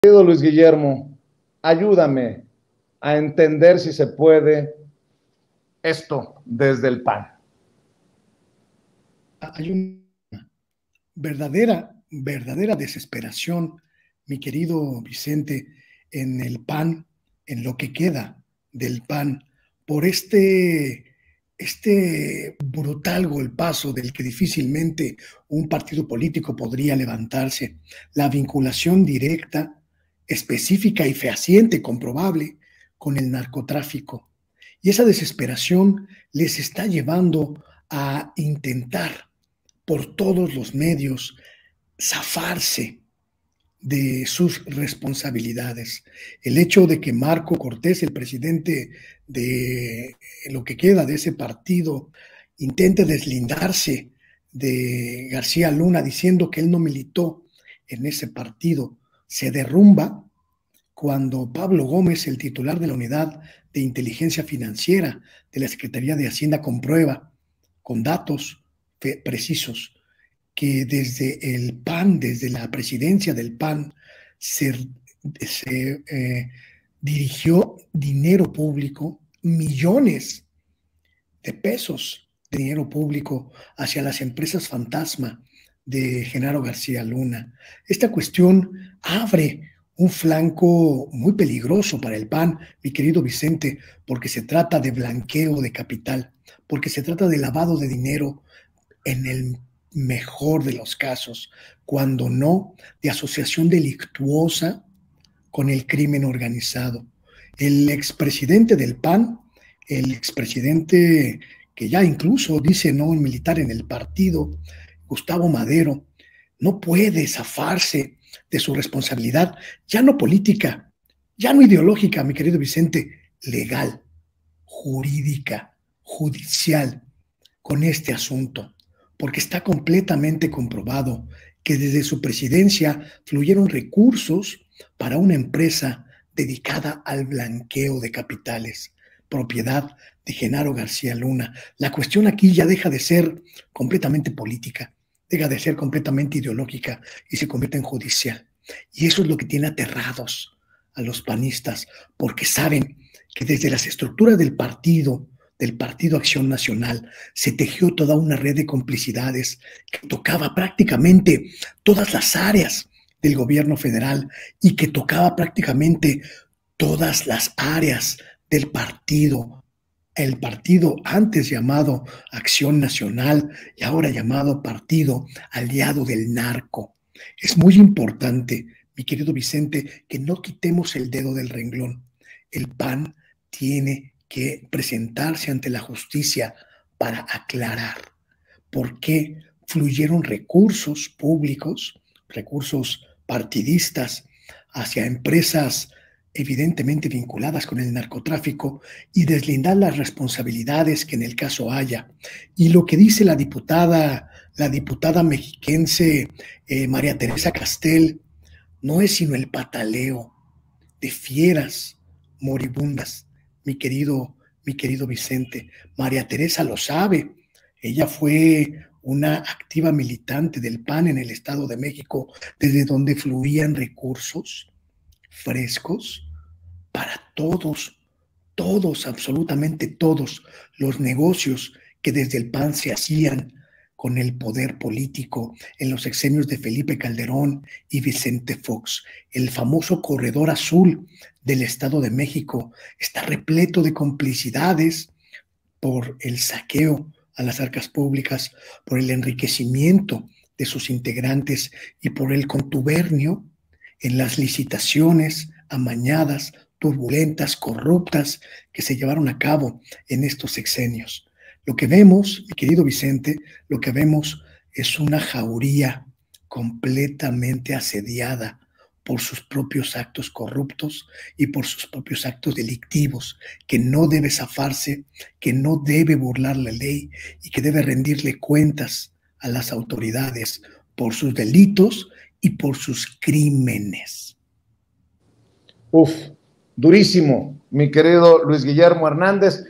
PAN ES UNA JAURÍA ASEDIADA POR SUS PROPIOS ACTOS CORRUPTOS: LG HERNÁNDEZ
El periodista libre Luis Guillermo Hernández nos plantea que la vinculación directa y comprobable del PAN con el n4rc0tr4f1c0 están llevando al partido a fracasar en sus intentos por defenderse absurdamente. Luis Guillermo aseguró que el PAN se está derrumbando ante el escándalo de una transferencia bancaria a una empresa de la red de corrupción de Genaro García Luna; comentó que Marko Cortés y Gustavo Madero ya no pueden zafarse del caso.